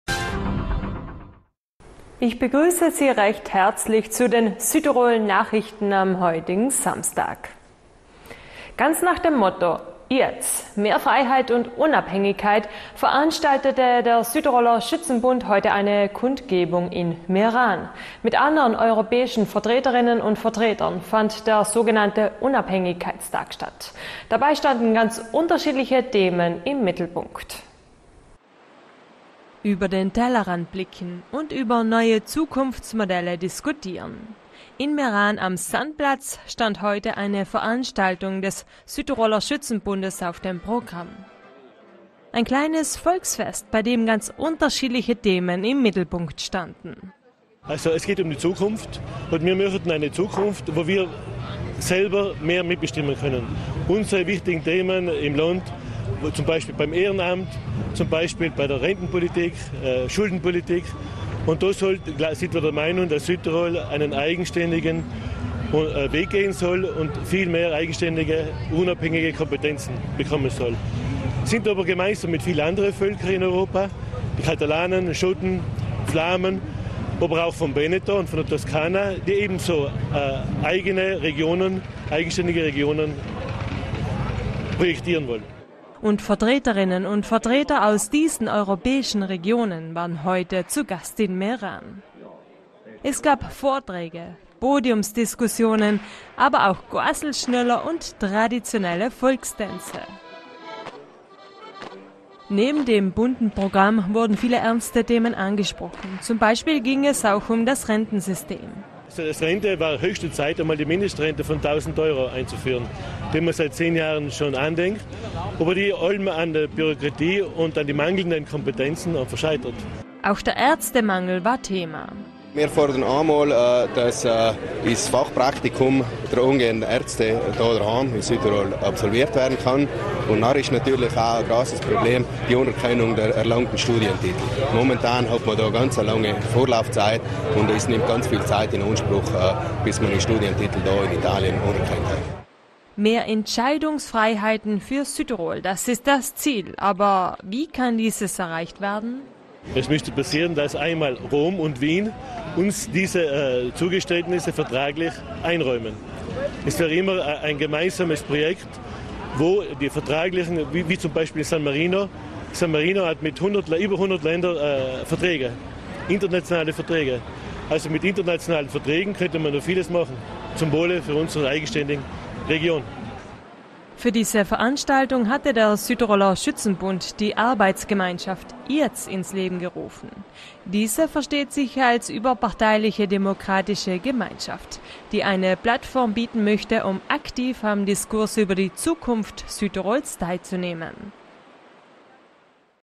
iatz-Tag in Meran - Bericht im SDF
Bericht im SDF vom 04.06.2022.

Es war ein Volksfest der besonderen Art, ein stimmungsvolles Fest für Jung und Alt und zugleich eine atemberaubende Willensbekundung in der ehemaligen Hauptstadt Tirols. Abgesehen davon, dass die Südtiroler kulturell und sprachlich überhaupt nicht zu Italien gehören, haben die Menschen erkannt, dass es auch ohne Italien geht. Und dass Italien die eigene Entwicklung bremst. Sie sind überzeugt, dass es der Weg der Unabhängigkeit ist, den es zu beschreiten gilt.